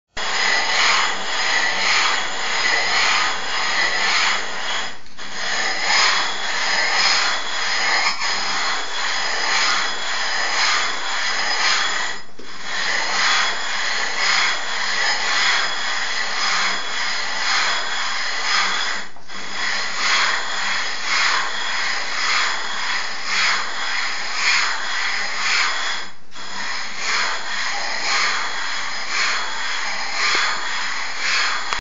Ebauchage de Tux
Première étape de la fabrication d'un miroir de télescope : l'ébauchage